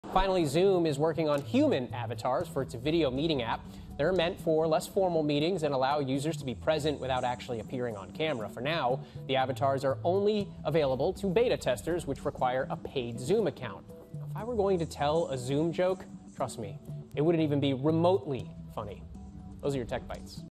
Zoom is working on 'human' avatars
The avatars are designed to let users be more present without having to be on camera.